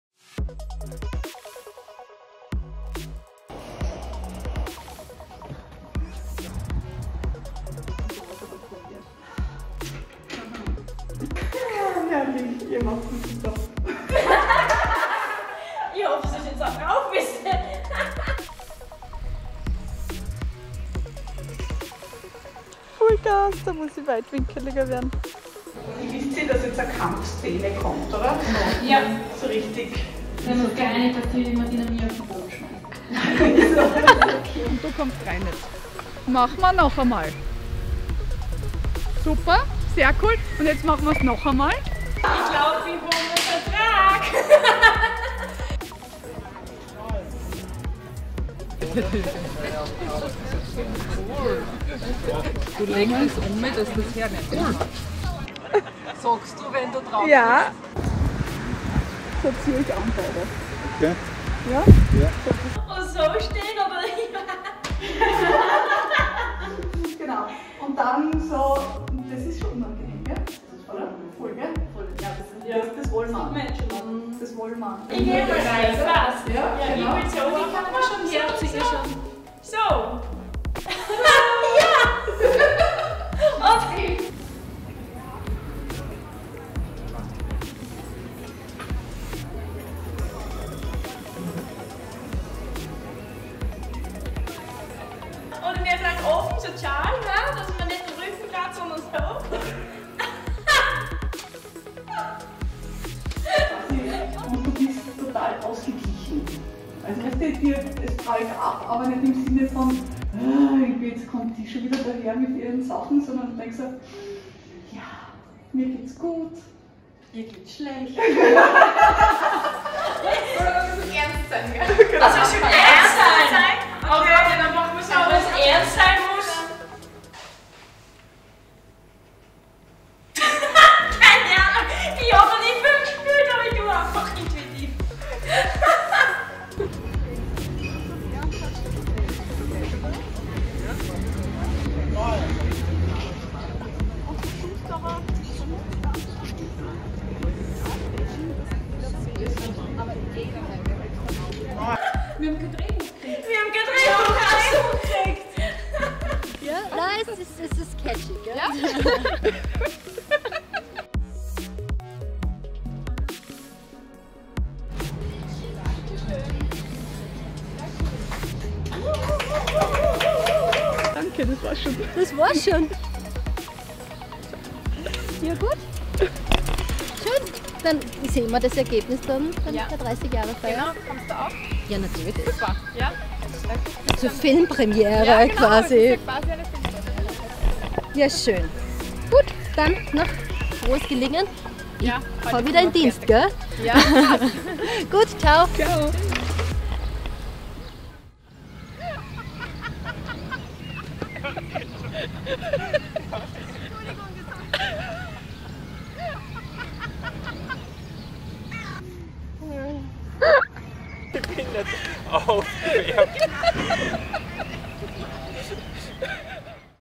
alpha nova Imagefilm: Hinter den Kulissen
Das Making- of zum neuen alpha nova Imagefilm.

Kamera: Ing. Astrid Rampula von astrimage FILM und Stefanie Pillip von alpha nova